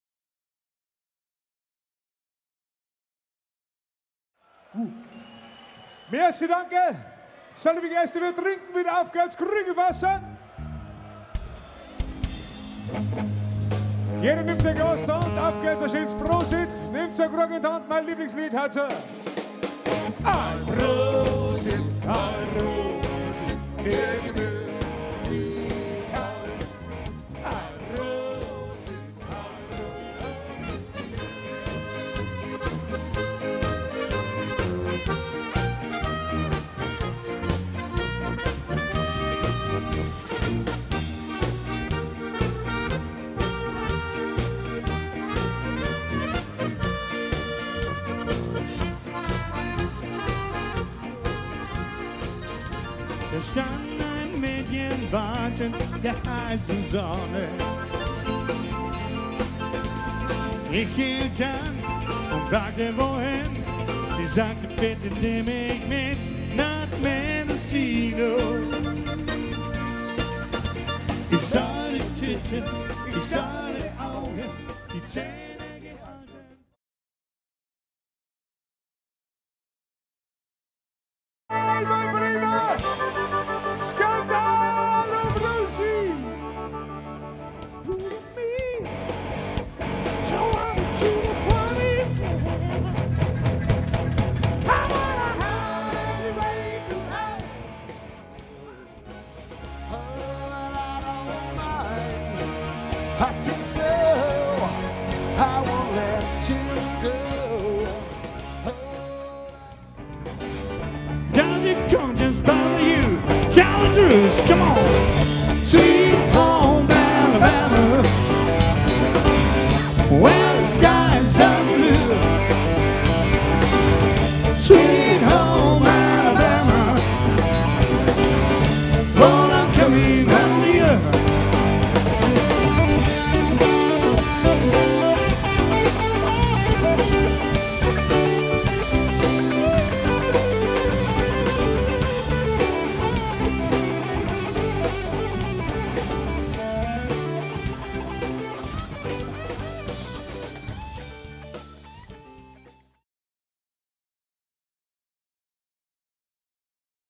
Simmisamma Trailer 1
Die beste Oktoberfestband der Welt räumt mit Vorurteilen auf